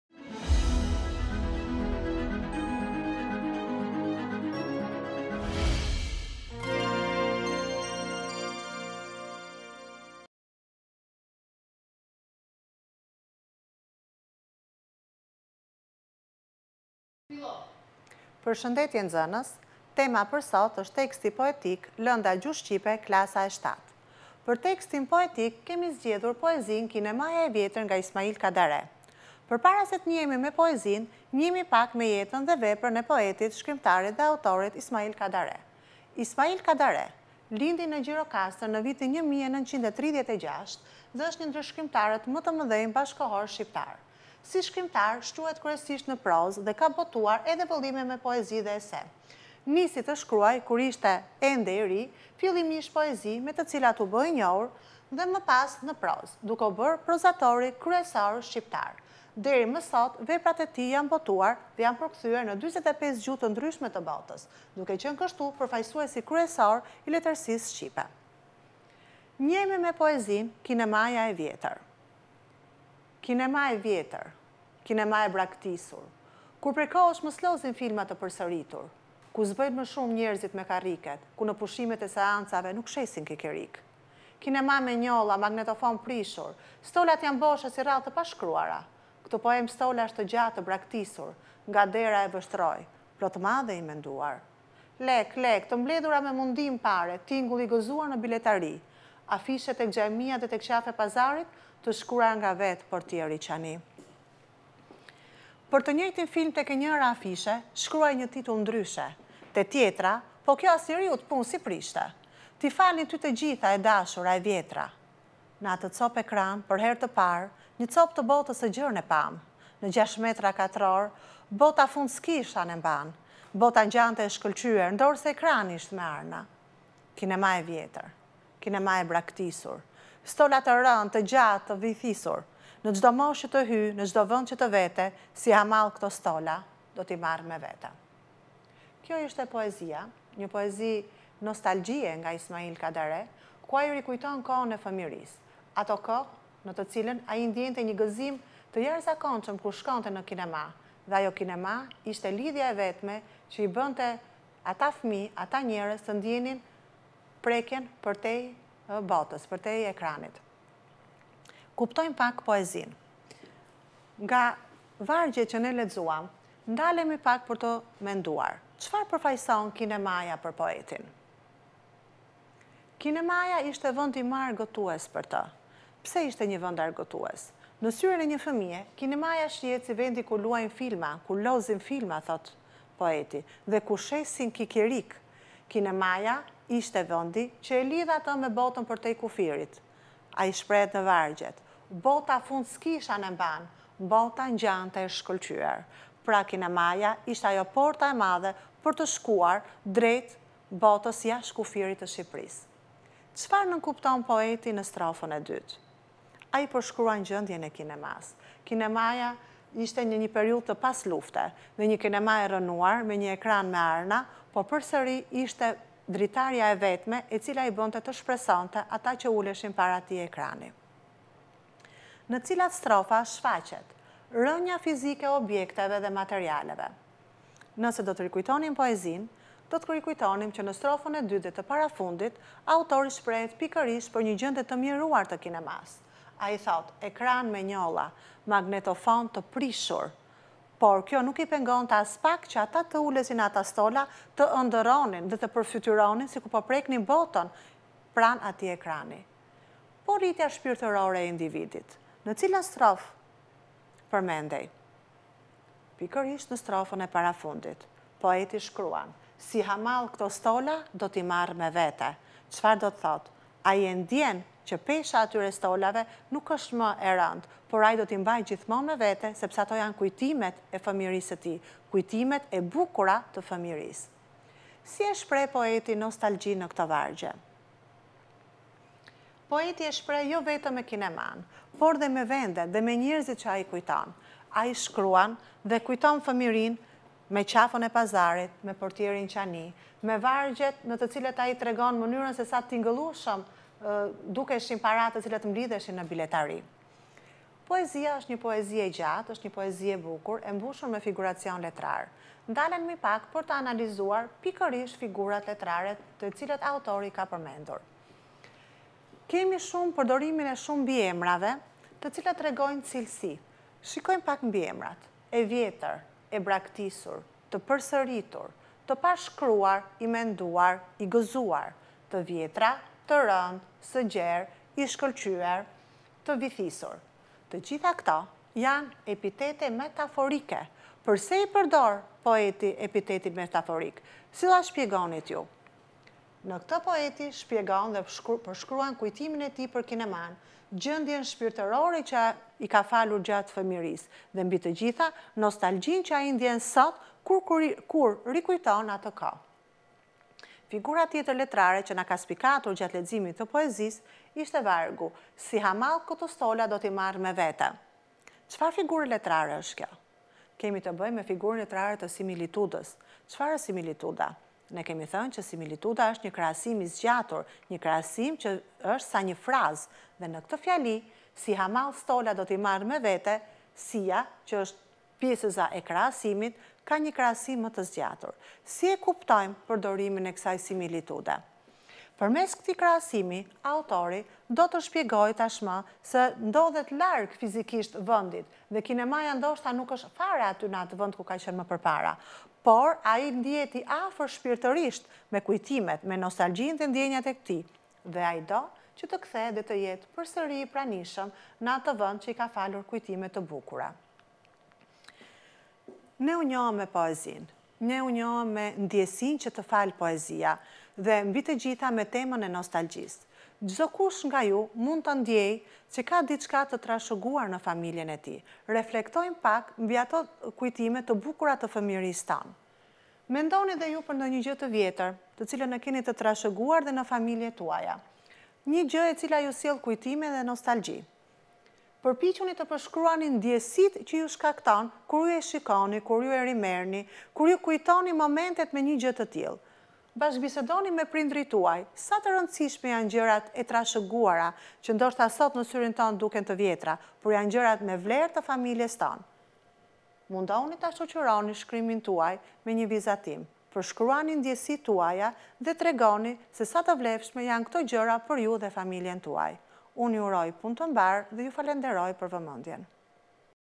Gjuhë Shqipe 7 - Tekst poetik “Kinemaja e vjetër”
Mësuese Suela Gjeçi